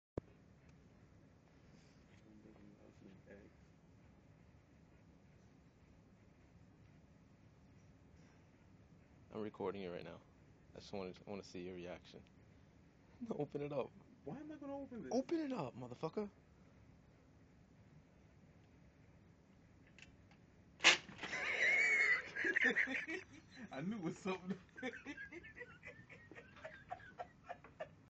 Rattlesnake Eggs - Joe.3GP
Joe gets scared by rattlesnake eggs.